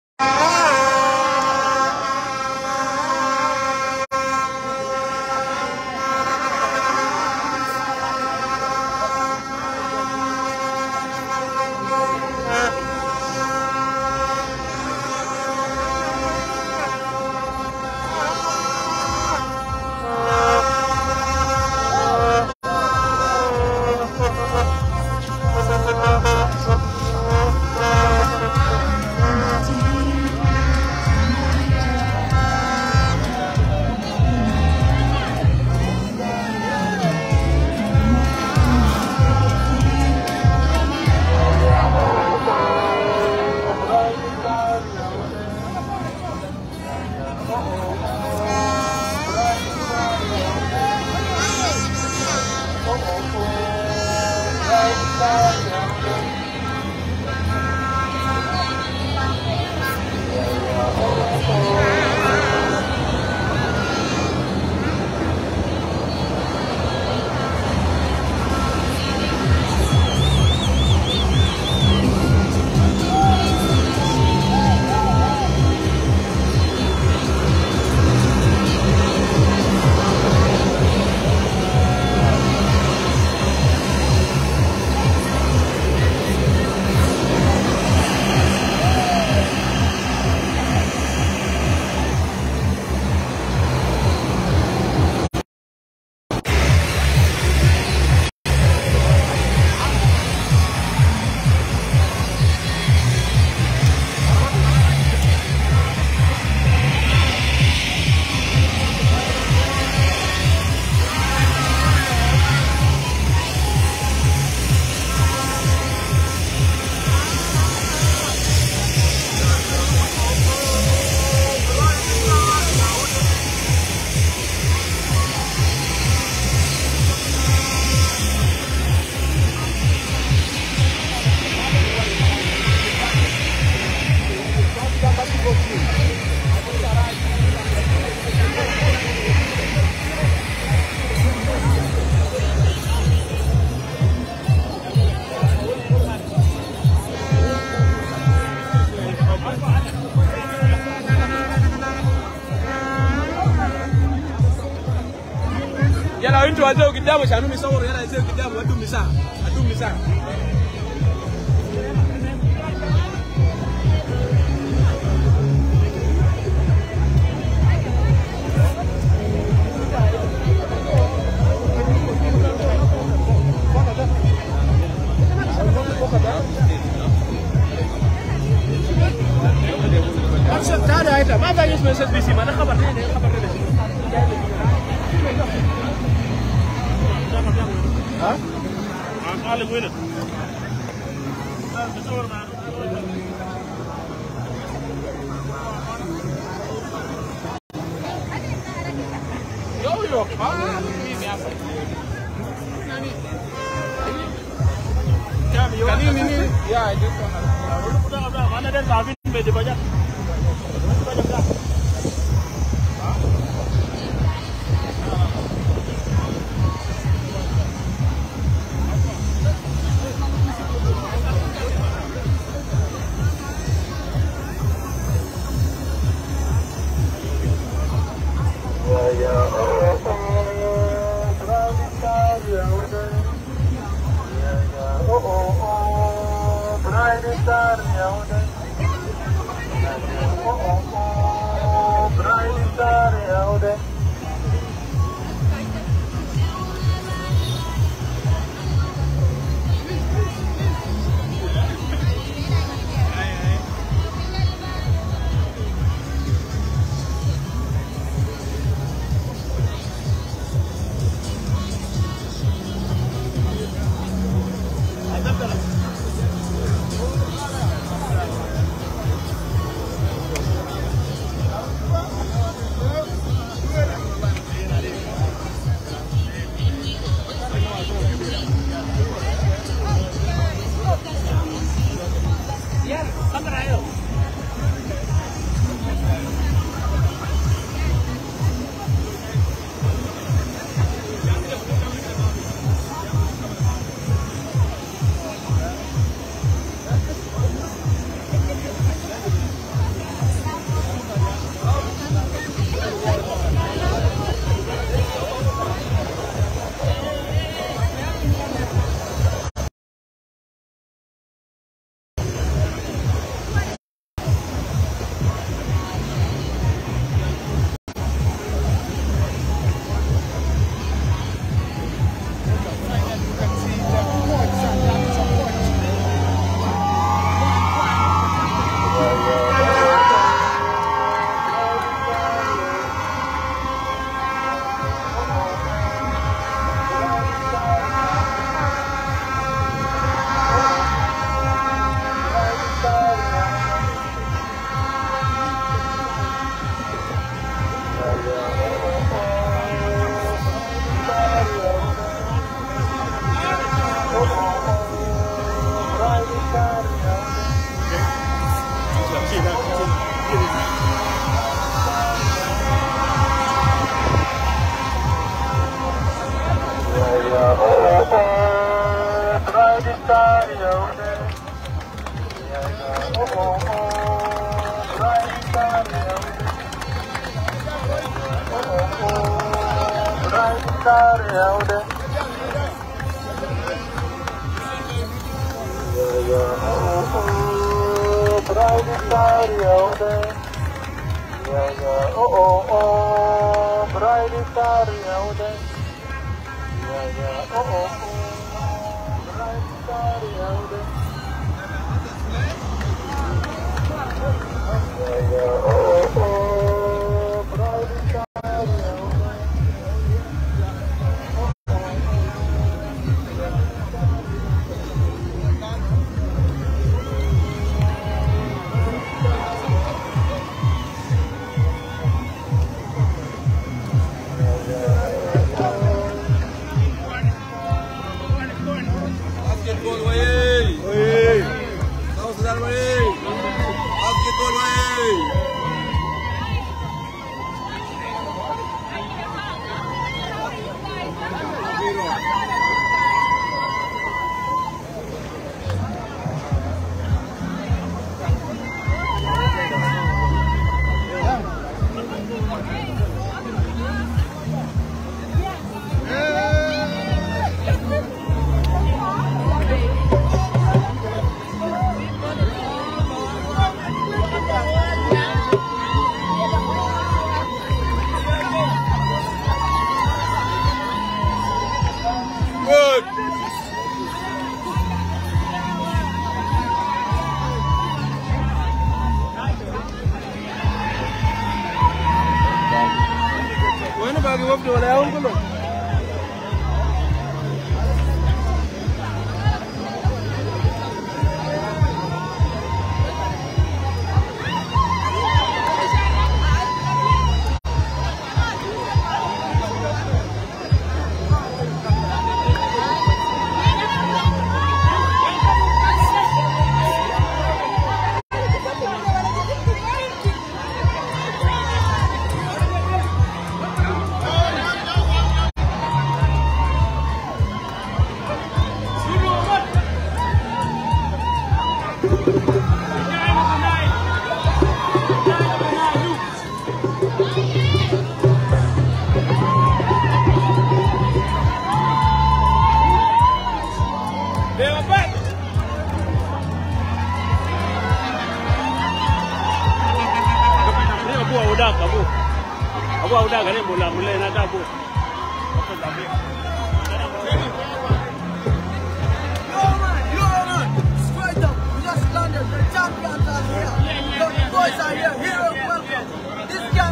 South Sudan Basketball Players Arrival at Juba International Airport
After Qualifying for FIBA Basketball World Cup in Egypt, South Sudan's National Basketball Team was welcomed in Juba at JIA.